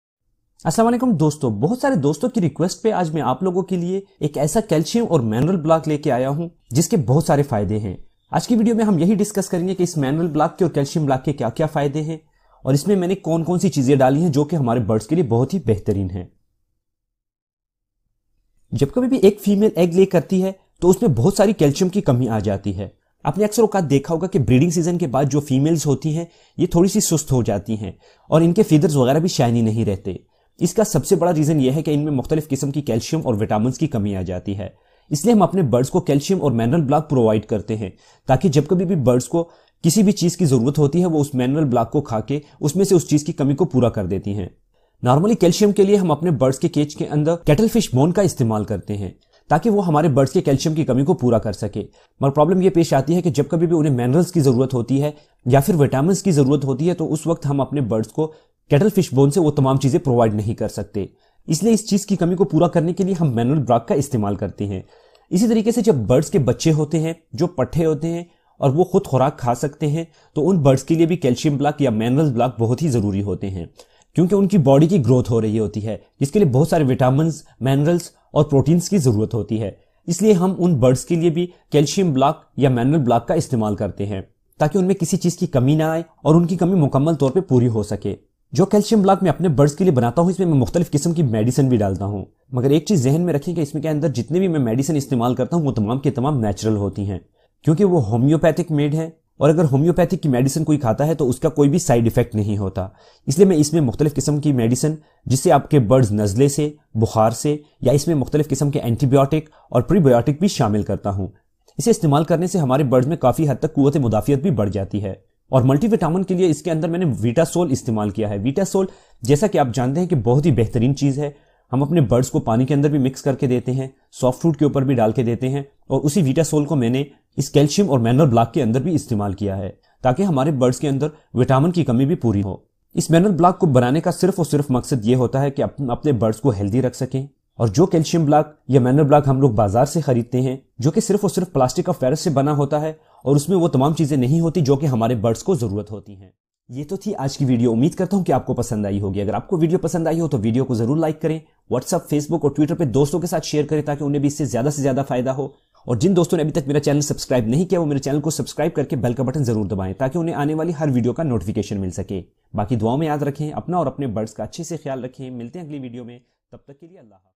Unique calcium block for birds
Sab ko pta hai birds ko calcium ki bohut zayda zarort hoti hai bazar say jo calcium block milty hain un main sirf plastr of pars hoti hai